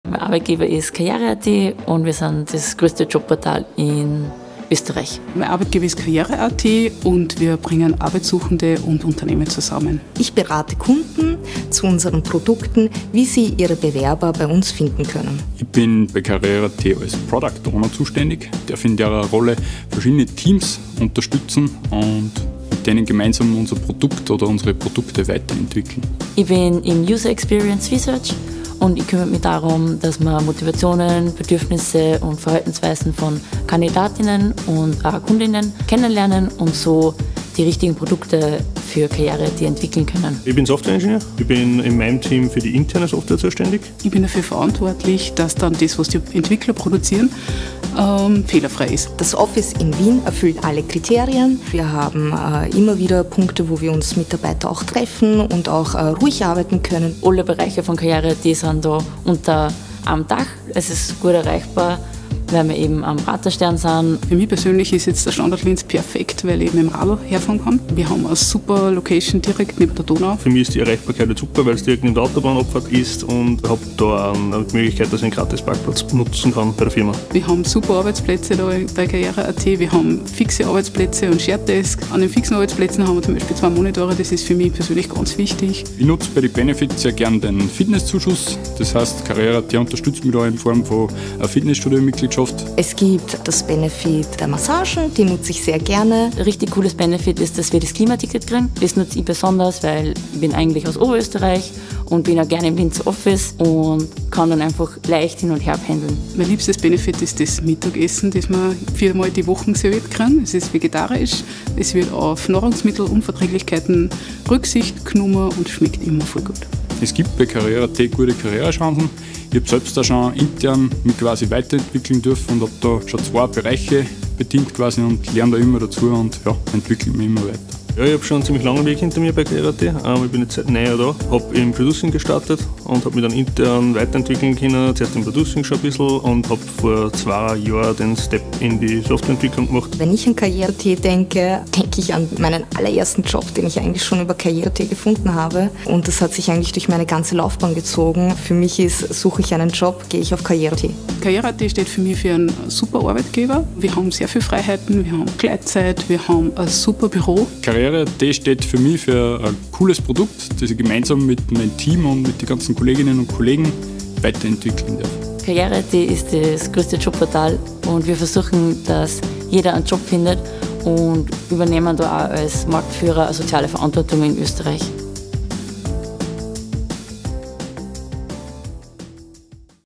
karriere.at als Arbeitgeber
Mitarbeiter erzählen, weshalb sie sich für karriere.at als Arbeitgeber entschieden haben.

Interview mit:
Jürgen Zeindlinger Software Engineer
Sharon Foschum  Software Test Engineer
Stefan Sigl   Product Owner
Susanne Raml  UX Researcher
Lorena Zorkic  Sales Manager